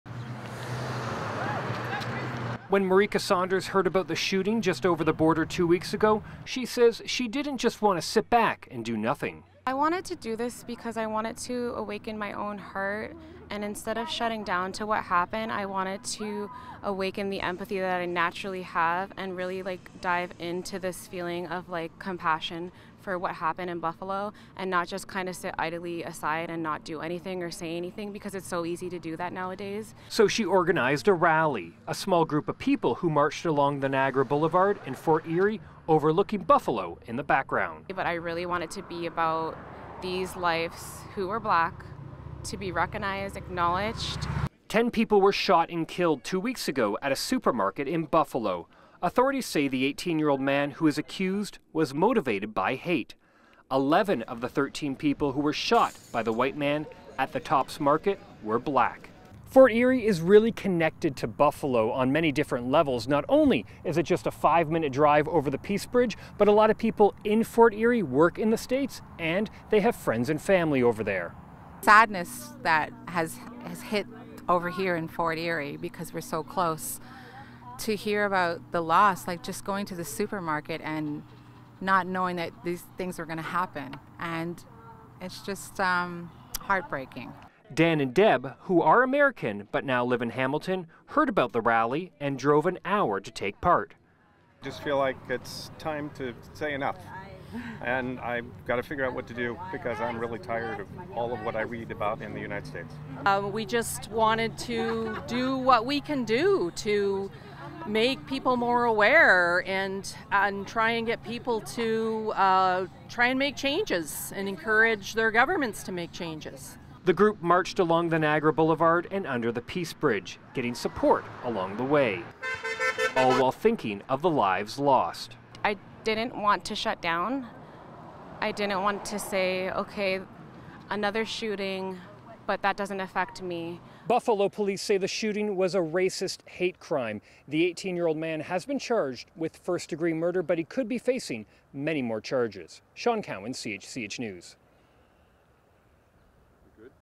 Community members in Fort Erie come together for march to remember Buffalo shooting victims
A young woman in Fort Erie organized a march today to remember the ten people that were shot and killed in Buffalo New York.

The mass shooting took place two weeks ago at a supermarket in what the authorities say was a racist hate crime.

Sean Cowan reports.